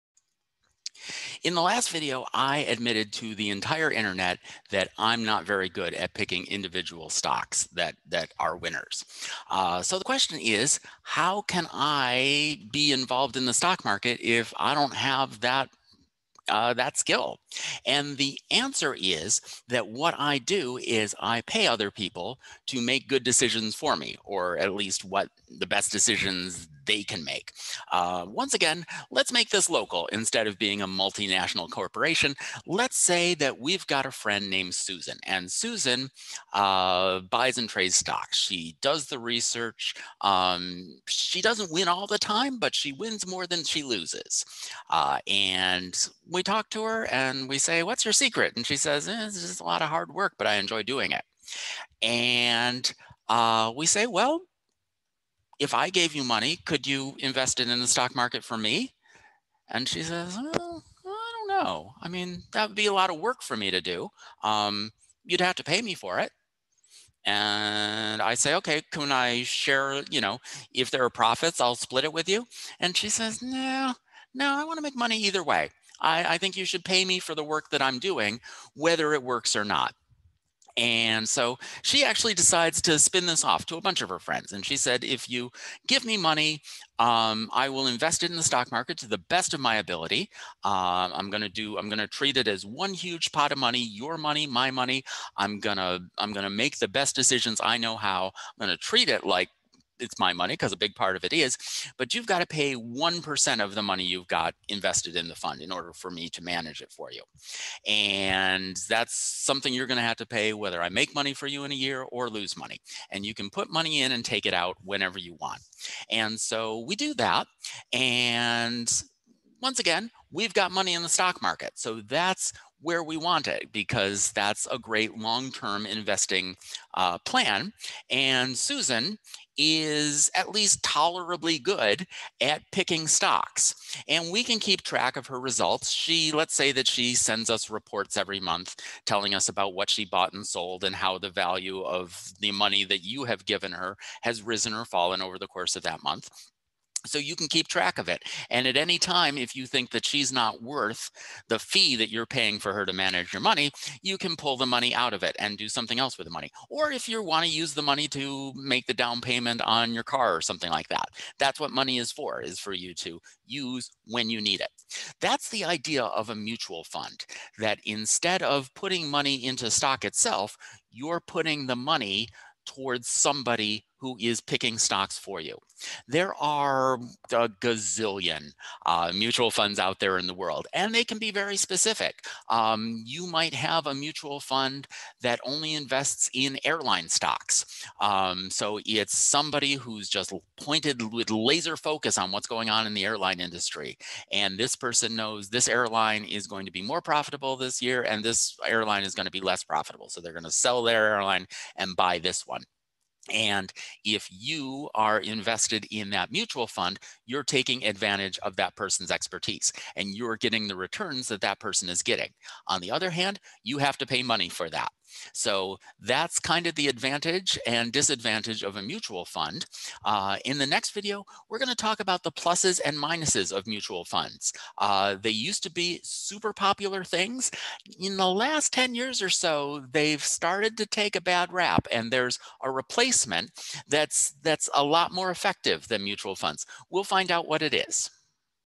Business Math - What is a mutual fund?
If you don't think that you'd be good at choosing your own stocks to invest in (and you would be right to be concerned about that), a mutual fund is a good way to combine diversification with good judgment.  This video describes how they work.